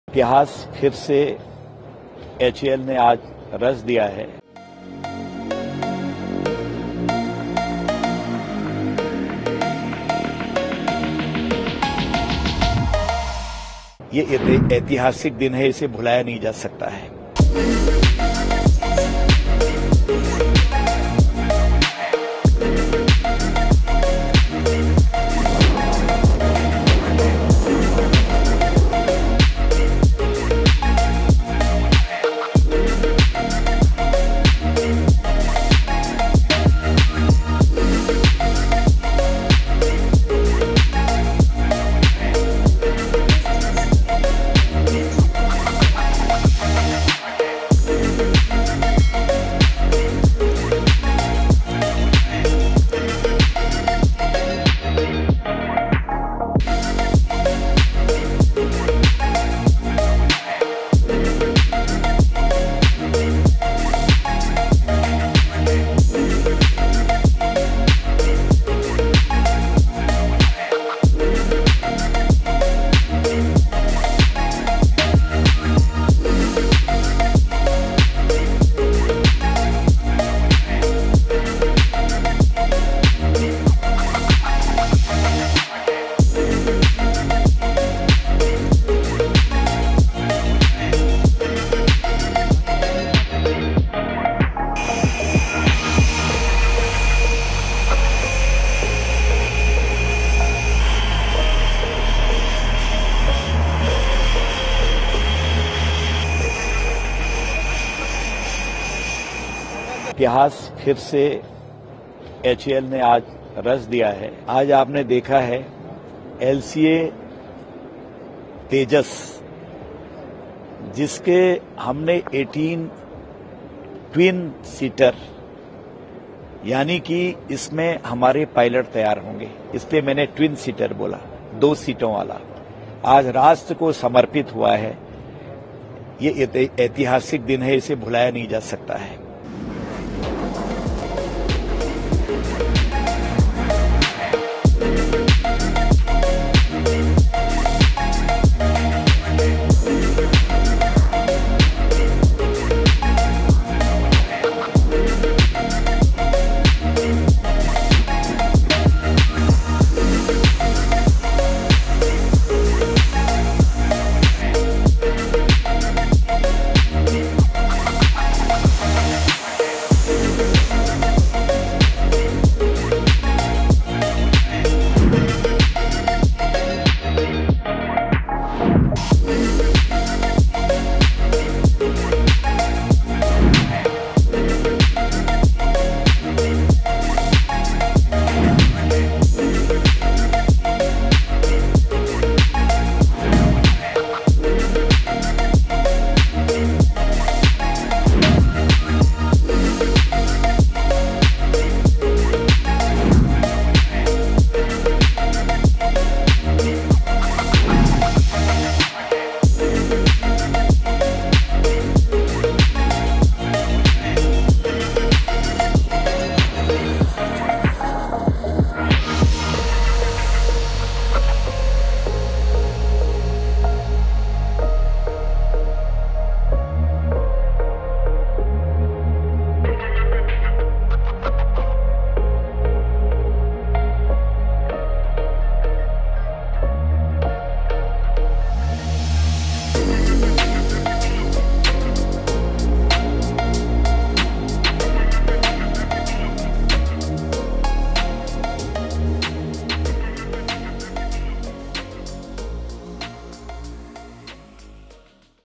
LCA Tejas trainer aircraft is in IAF’s arsenal; what makes LCA twin-seater aircraft so special?
Hindustan Aeronautics Limited handed over the first LCA Tejas twin-seater trainer version aircraft to IAF on Oct 04. MoS for Defence Ajay Bhatt, Air Chief Marshal VR Chaudhari were present at the LCA Tejas division plant. This historic event mark the achievement of a key milestone of producing LCA Twin Seater. It is designed with a strategic intent of graduating budding pilots from the Twin Seater variants to fighter Pilots. HAL has an order of 18 twin seater from IAF and is planning to deliver eight twin seater in FY 2023-24. Further, 10 Light Combat Aircraft twin seaters would be delivered progressively by 2026-27. LCA-Tejas is the largest R-D program ever undertaken in India that made its maiden flight in 2001. It is going to be the backbone of the IAF fleet in years to come and is slated to be produced in high numbers. HAL has already received an order for 123 aircraft out of which 32 fighters have been supplied to IAF. This adds India to the list of elite countries that have created such capabilities in their Defence Forces. LCA Tejas is the best platform for India to be a flag bearer to achieve export targets set by the government.